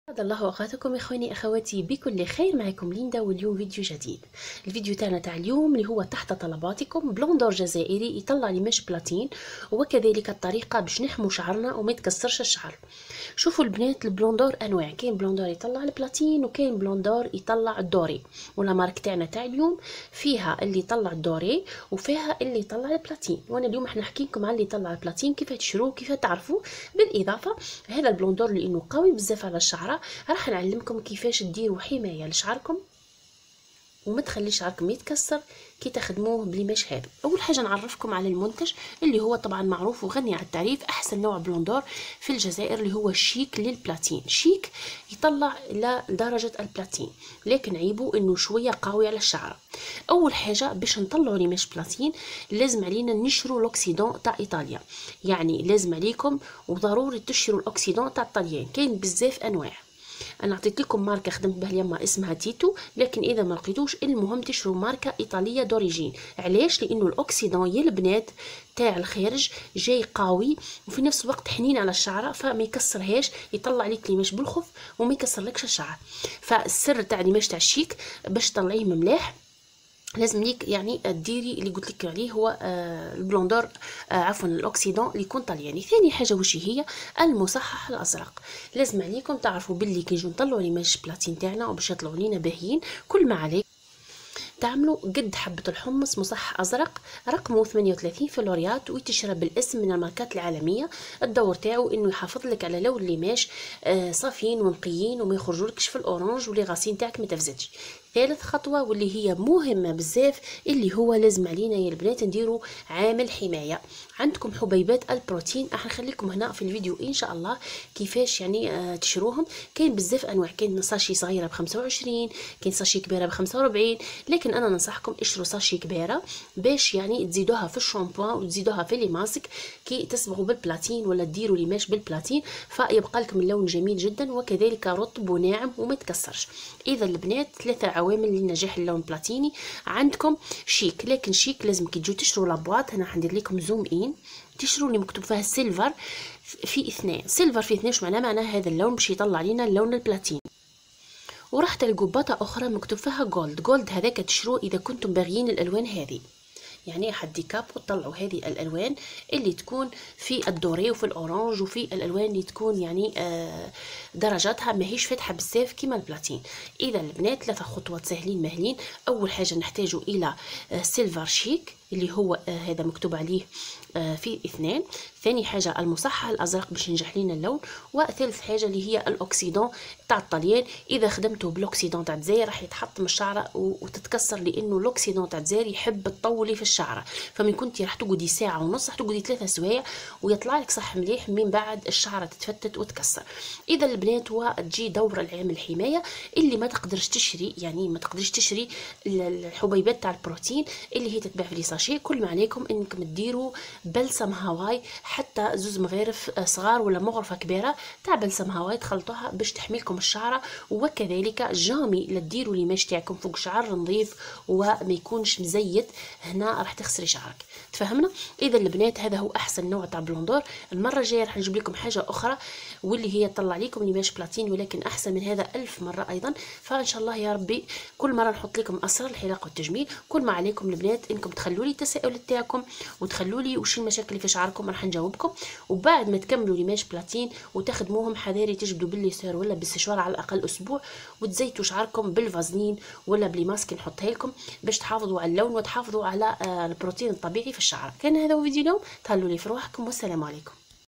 أفضل انواع البلوندور🇩🇿 المتواجده في السوق والافضل للحصول على لوم البلاتين😊لي تزربلنا الخدمه
افضل انواع البلوندور المتواجده في السوق والافضل للحصول على لوم البلاتين☺لي تزربلنا الخدمه🤗